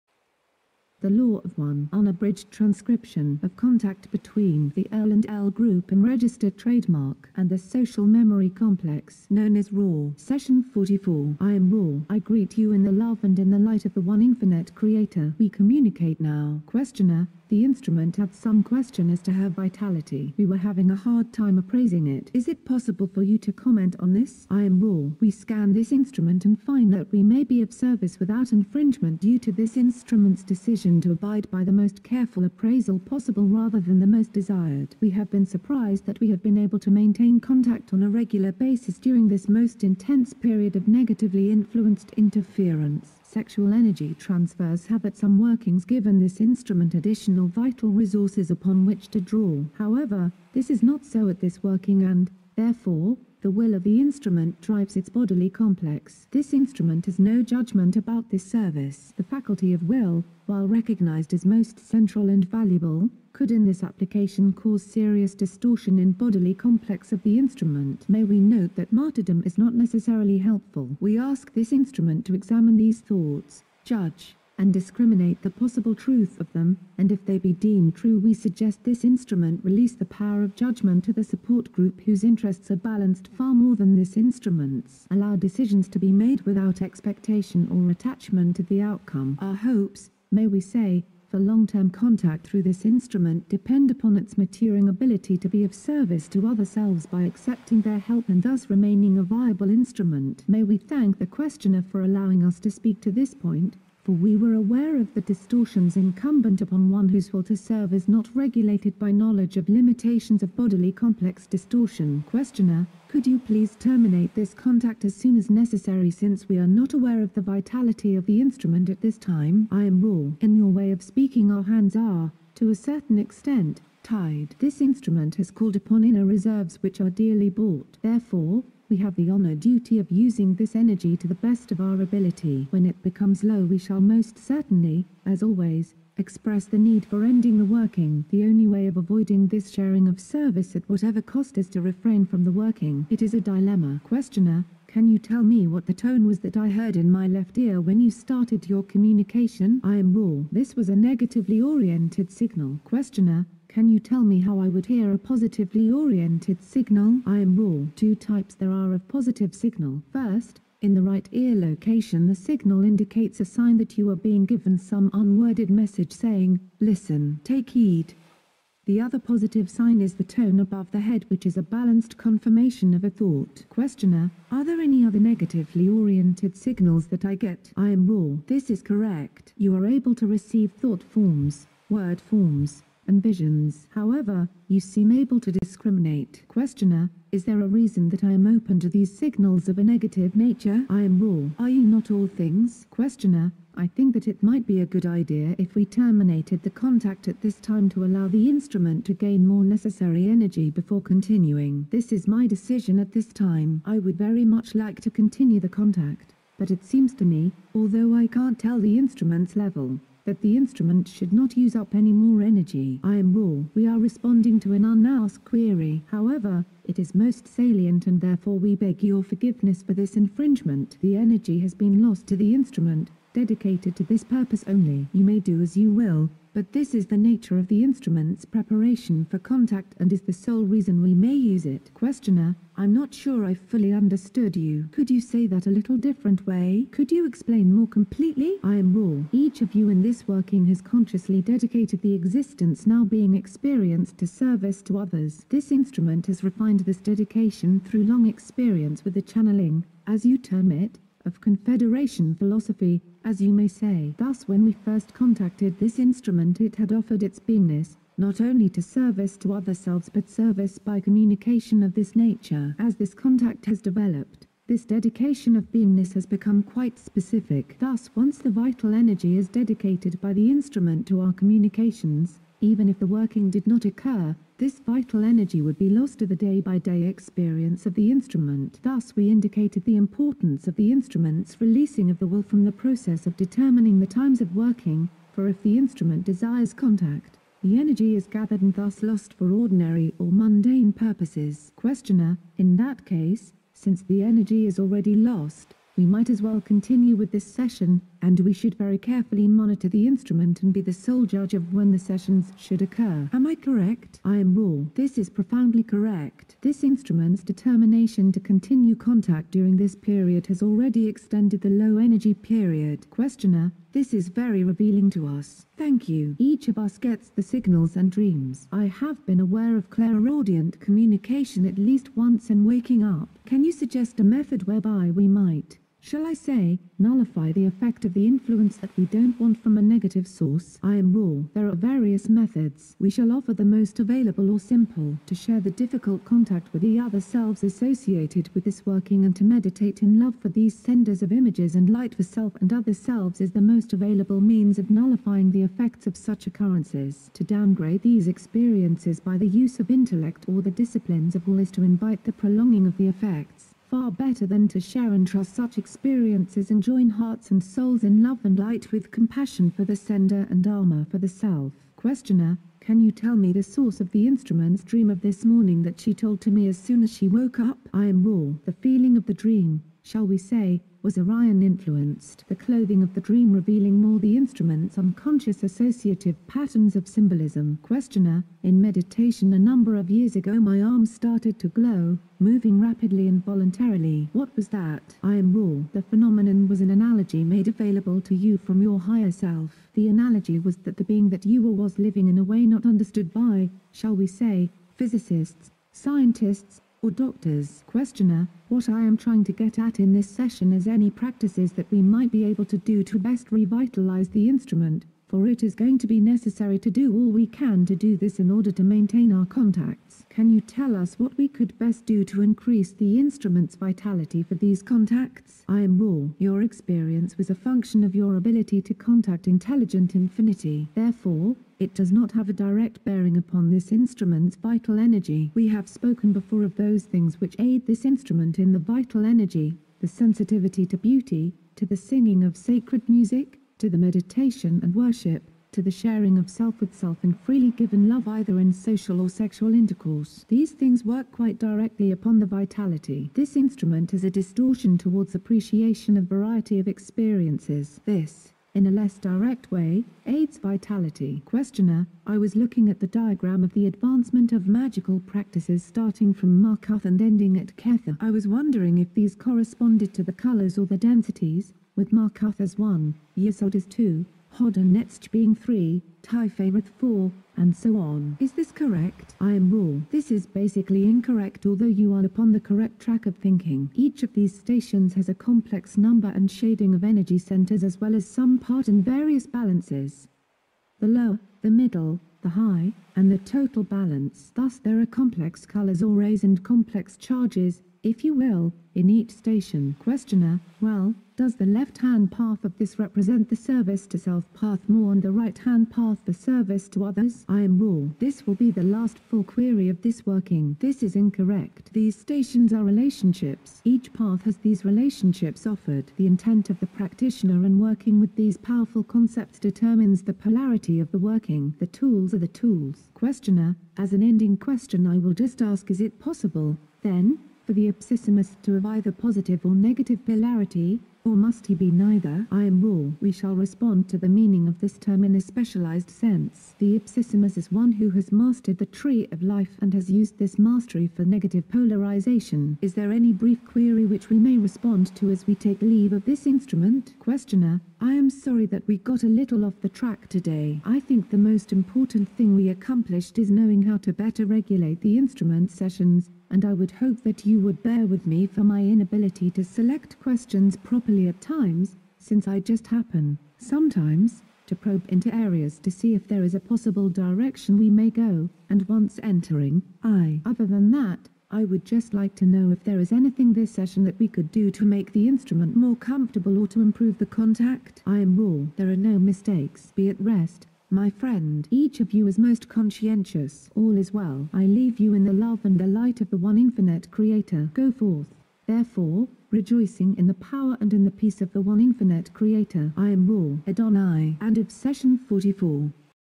The Law of One - Session 044 - The Ra Material
Session 044: Positive and Negative Entity Contact Signals, Polarity in Magical Practice.
The Law of One • The Ra Material
Don Elkins, Carla Rueckert, Jim McCarty.